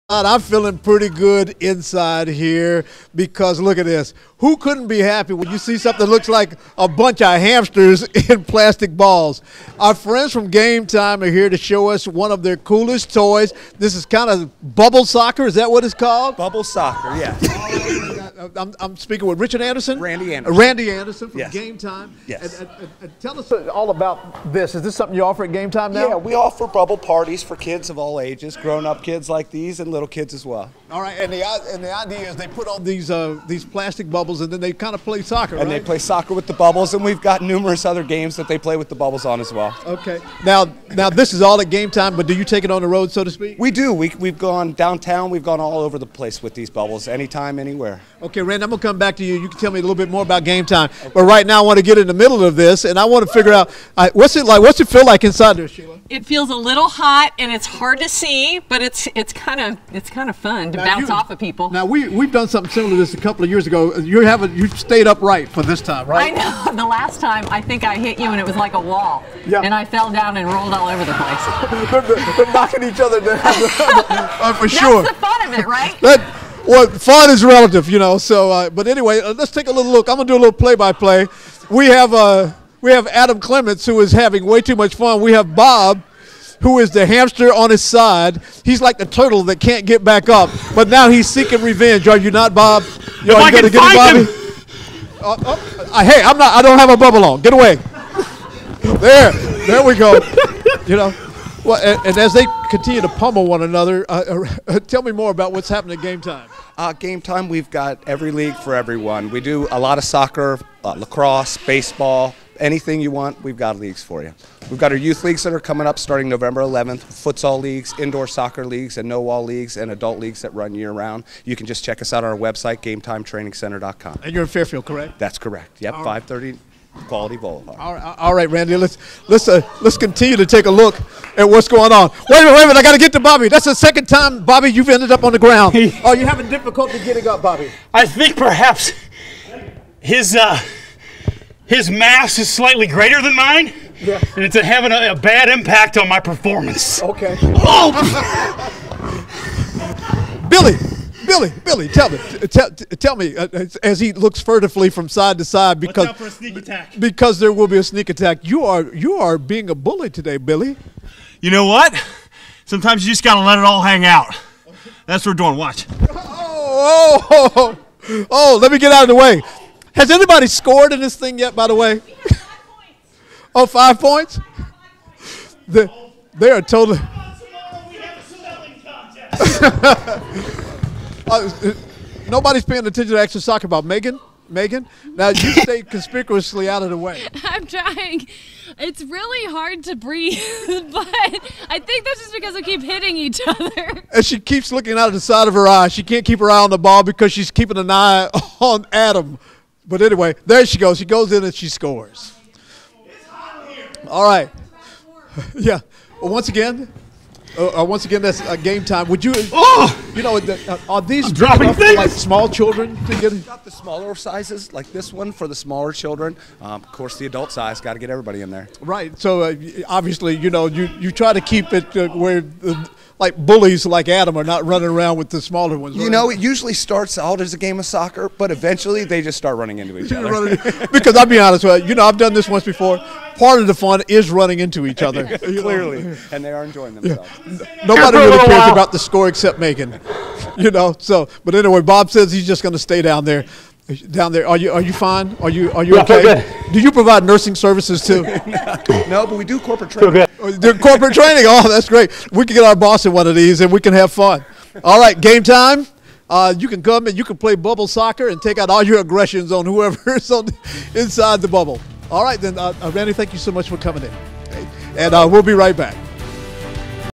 GMC crew plays GameTime's bubble soccer
CINCINNATI (WKRC) - It looks like a bunch of hamsters in plastic balls. Our friends from GameTime Training Center showed off one of their coolest games - bubble soccer.